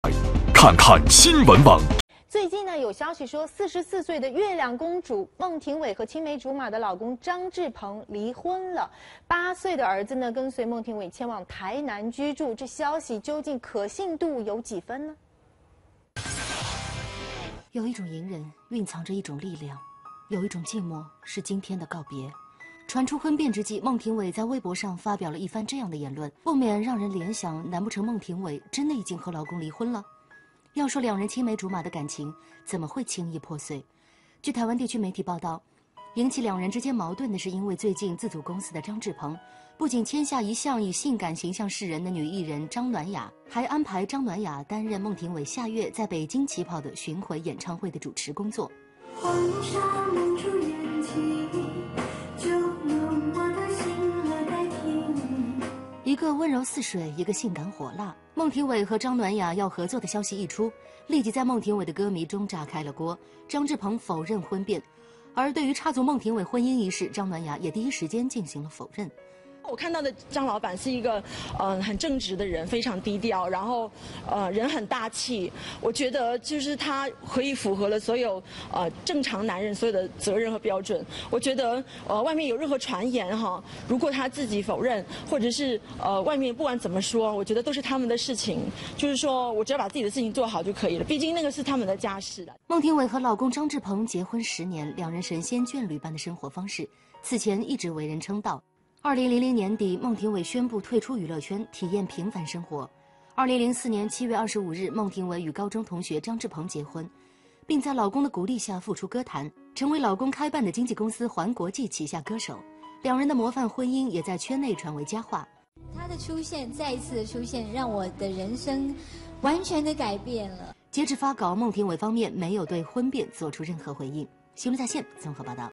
性感女星插足？44岁孟庭苇Mai被曝已离婚 老公承认吵架否认婚变
44岁歌手孟庭苇惊传与结婚10年的高中青梅竹马张志鹏离婚！她是他开设经纪公司"洹国际"旗下歌手，读者爆料2人去年早已离婚，8岁儿子目前跟孟庭苇住台南；2人微博、脸书已许久都各自po文贴图，未见全家福。对此，张志鹏昨称"在工作上会吵架"，但否认离婚，并称儿子因已上小学才不再频繁露脸；形象端庄的孟庭苇下月2日起将展开巡回演唱会，主持人是走性感路线的张暖雅，也引起歌迷反弹。